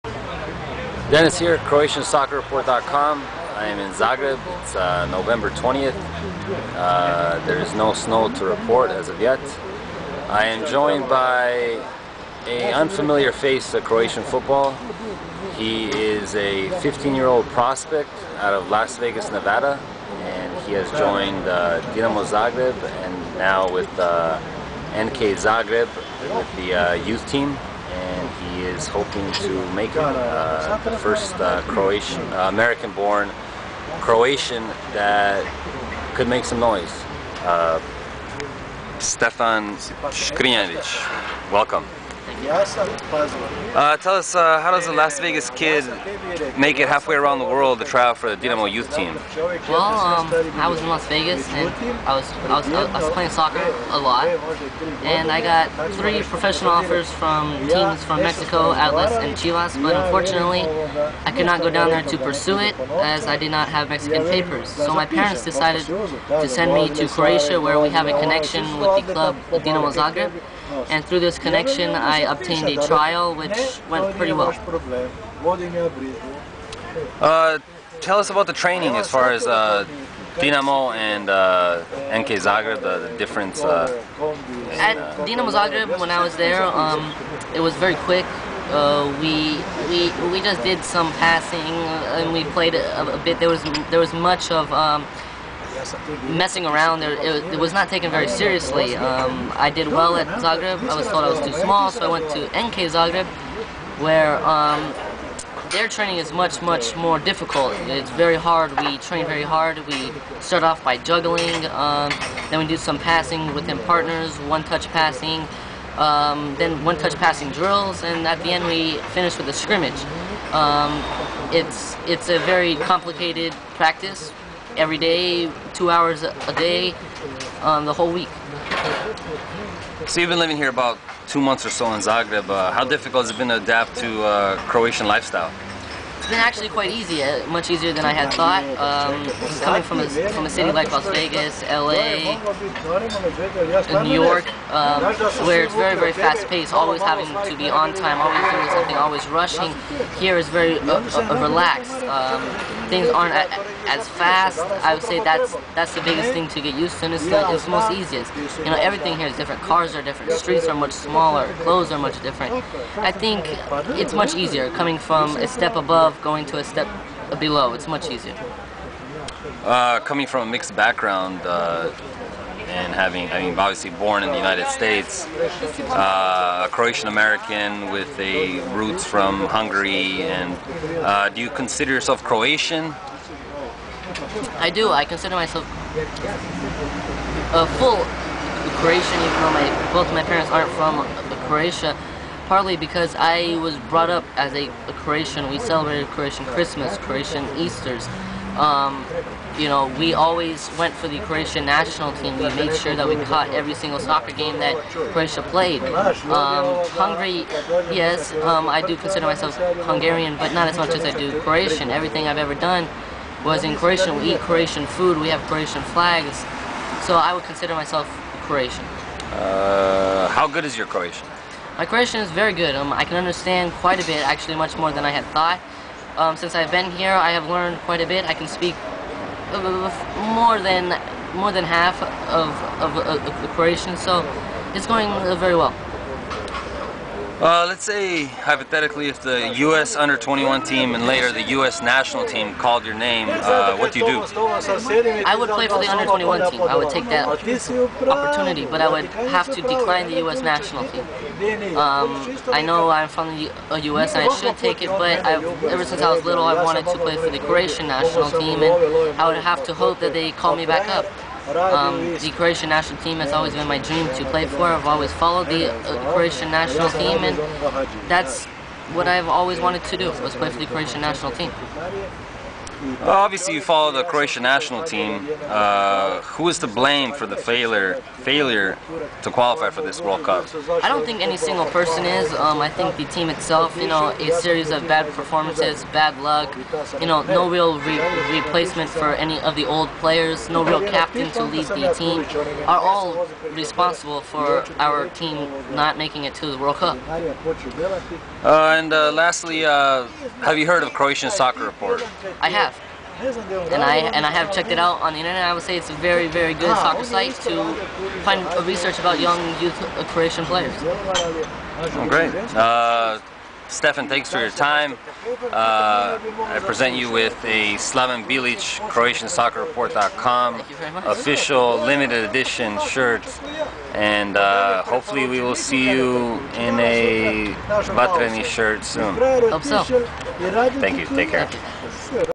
Stefan Škrinjarić NK Zagreb, Dinamo Zagreb
15 year old prospect out of Las Vegas Nevada, Stefan Škrinjarić trains with NK Zagreb and  Dinamo Zagreb in hopes of making it in Croatian football